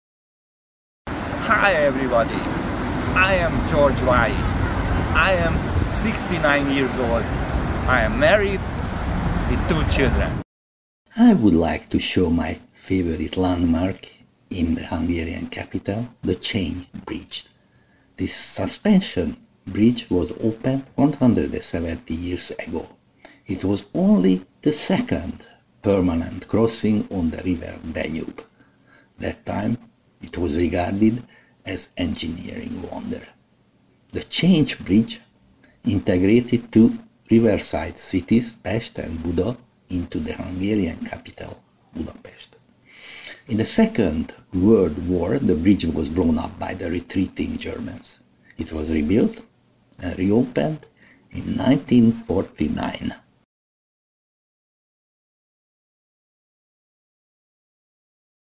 Chain Bridge 1849-2019, Budapest, Hungary
Very short history of the Szechenyi Chain Bridge from 1849 until 2019.
Created by George Valyi for Google Connect Live 2019.
Budapest, March 2019.